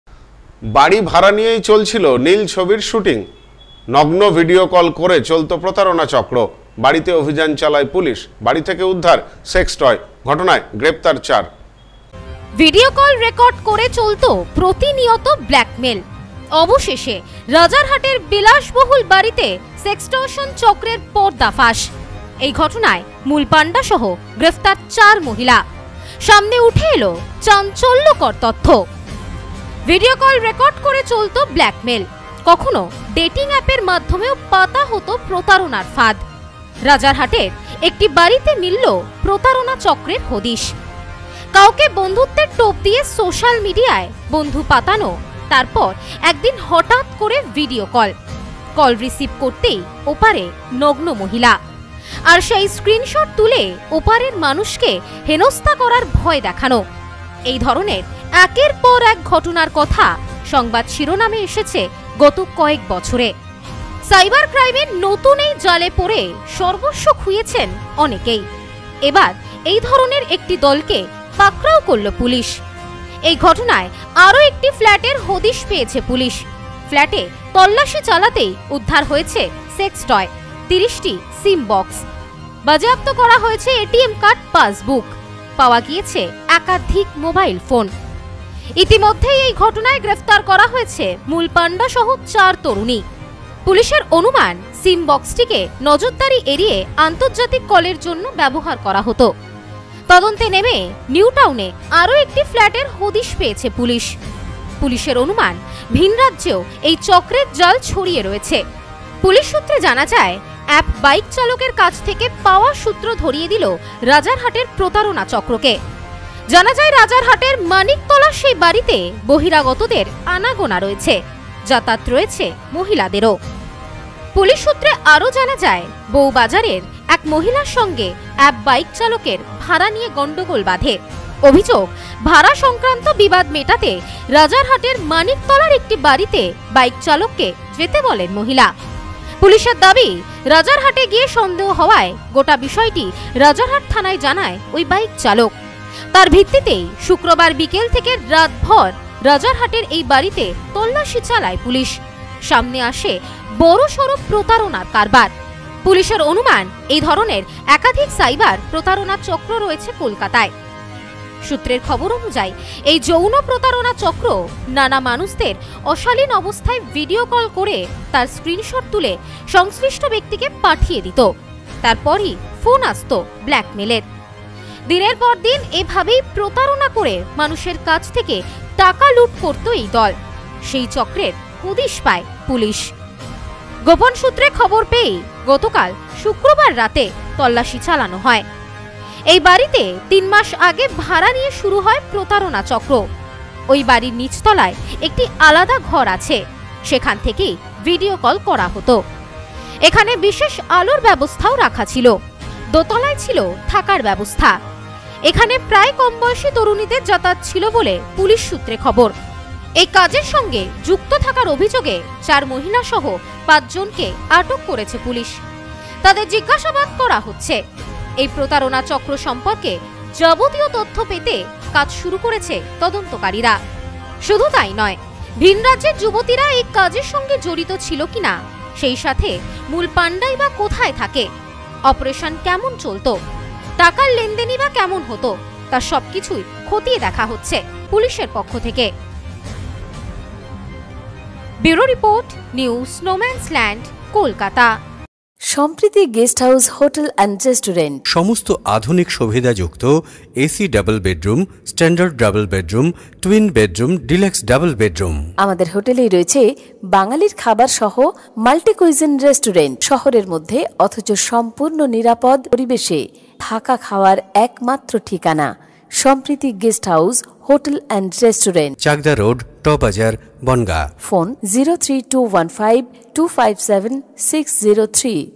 বাড়ি ভাড়া নিয়ে নীল ছবির শুটিং! নগ্ন ভিডিও কল করে প্রতারণা
বাড়ি ভাড়া নিয়ে নীল ছবির শুটিং! নগ্ন ভিডিও কল করে প্রতারণা, বাড়িতে তল্লাশি চালিয়ে উদ্ধার সেক্সটয়, গ্রেপ্তার ৪।